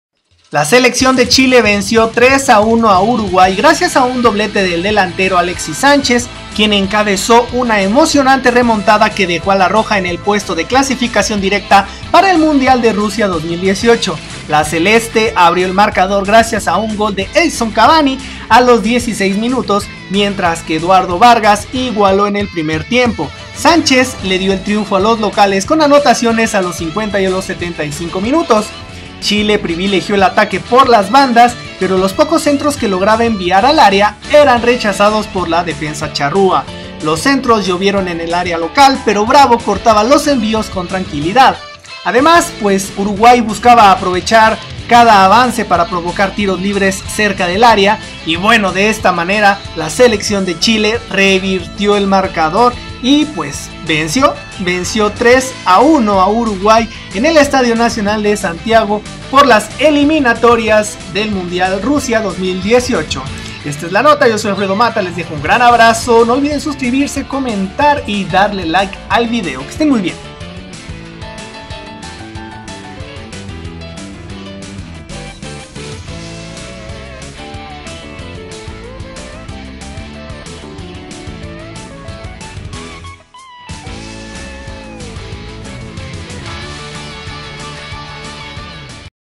Chile Golea 3-1 a Uruguay
La selección Chile venció 3-1 a Uruguay gracias a un doblete del delantero Alexis Sánchez, quien encabezó una emocionante remontada que dejó a "La Roja" en puesto de clasificación directa para el Mundial 2018. La "Celeste" abrió el marcador gracias a un gol de Edinson Cavani a los 16 minutos, mientras que Eduardo Vargas igualó en el primer tiempo. Sánchez le dio el triunfo a los locales con anotaciones a los 50 y 75. Chile privilegió el ataque por las bandas pero los pocos centros que lograba enviar al área eran rechazados por la defensa "charrúa". Los centros llovieron en el área local pero Bravo cortaba los envíos con tranquilidad. Además, Uruguay buscaba aprovechar cada avance para provocar tiros libres cerca del área. Y bueno de esta manera la selección Chile revirtió el marcador. Venció 3-1 a Uruguay en el Estadio Nacional de Santiago por Eliminatorias.
Así jugaron:
Chile: Bravo; Isla, Medel, Jara, Beausejour; Hernández, Vidal, Díaz; Sánchez, Vargas, Fuenzalida.
Uruguay: Muslera; Pereira, Godín, Coates, Silva; Arévalo Ríos, Vecino, Sánchez, Gonzalez, Suárez, Cavani.
- HEEY!! LA LISTA COMPLETA DE NOTICIAS DEPORTIVAS

~ Alfredomatta